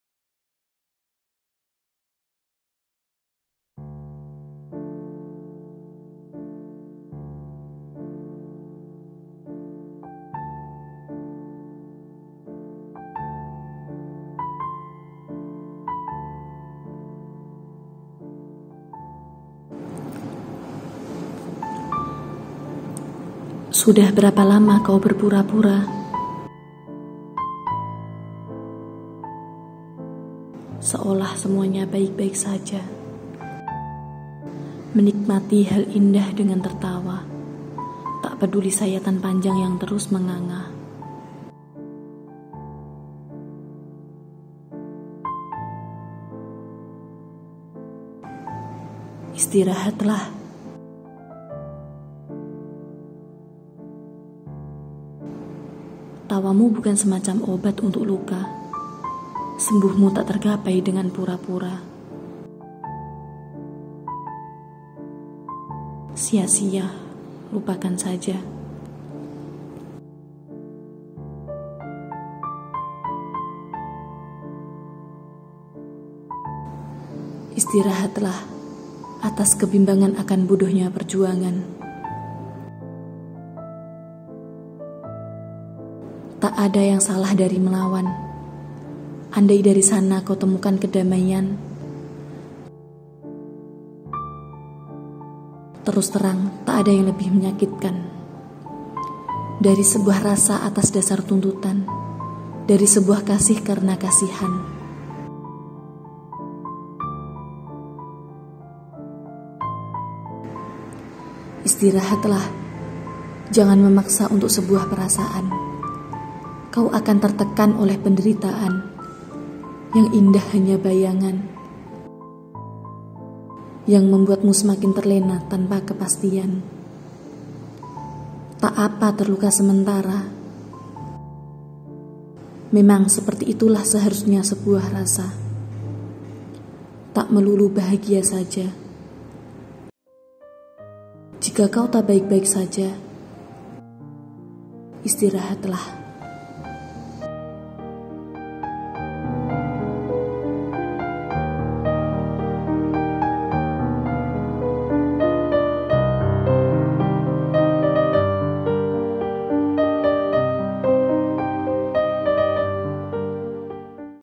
ISTIRAHATLAH - VISUALISASI PUISI
A poem by Aliven

Sebuah puisi yang dipersembahkan untuk seluruh hubungan di dunia ini, yang seharusnya berakhir namun tak sempat mengakhir.

Location : Warkop Warmindo 180 E-Sport